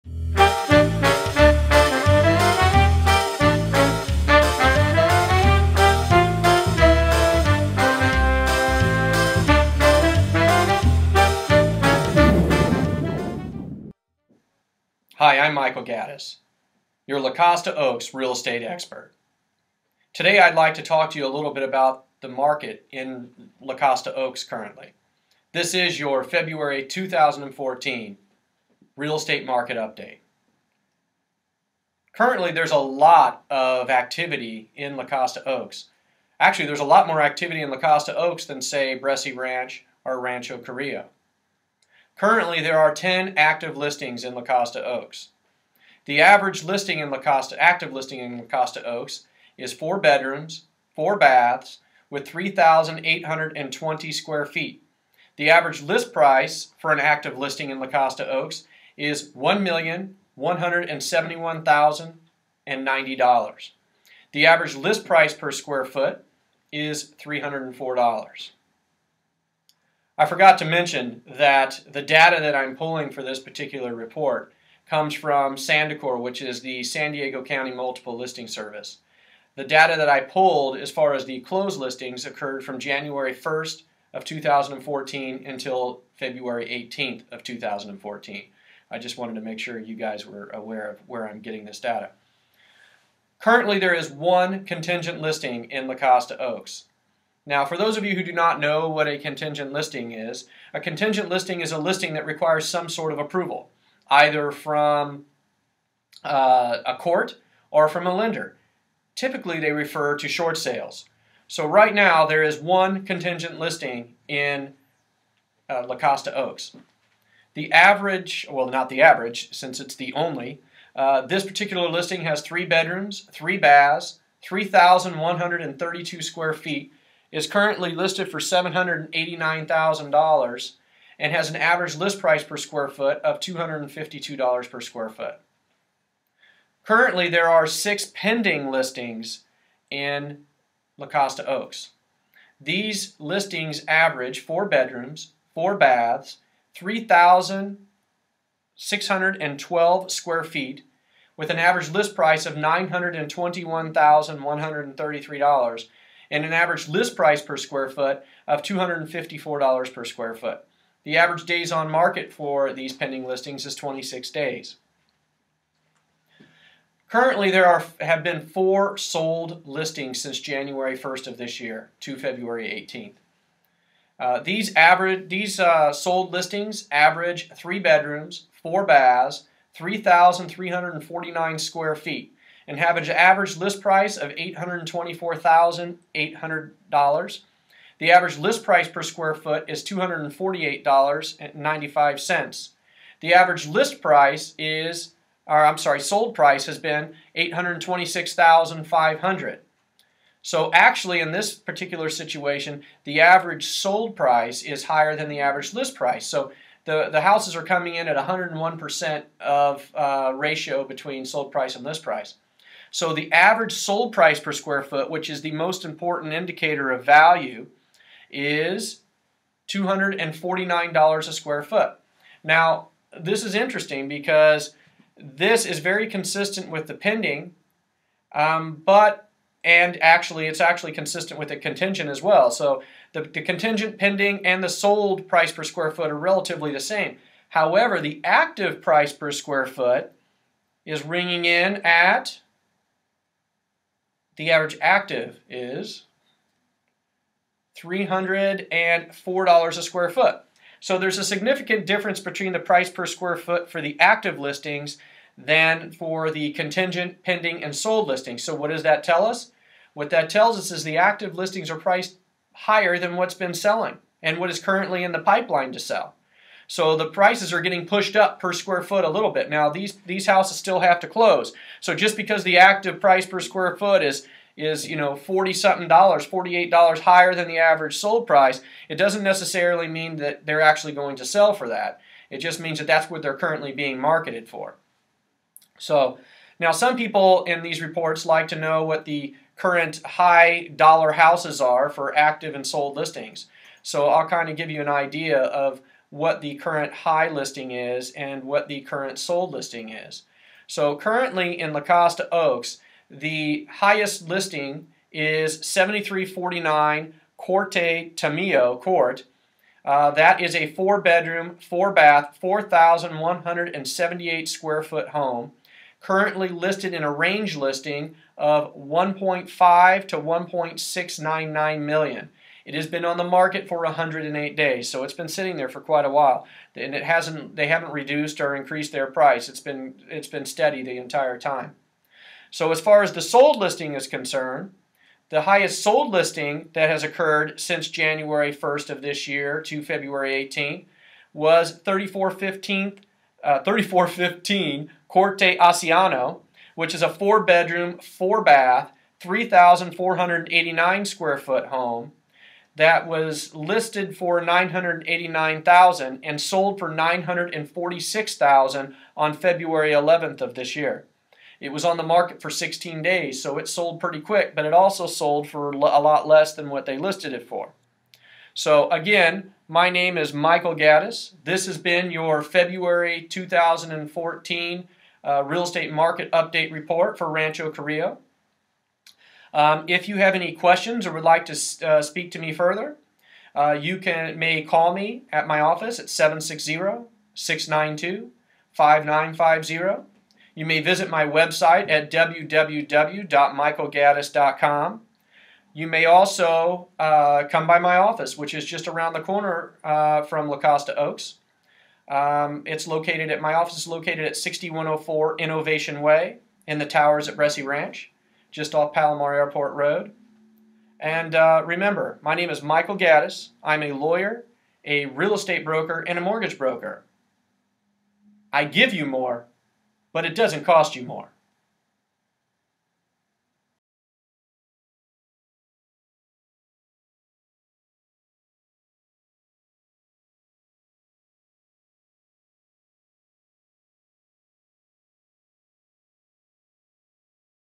La Costa Oaks Homes for Sale & Real Estate Update for February 2014
La Costa Oaks Real Estate specialist, Michael Gaddis, shares market data for the La Costa Oaks neighborhood, from the Sandicor MLS. Discover what homes are selling for, how many homes are listed as well as the average cost per square foot in the area.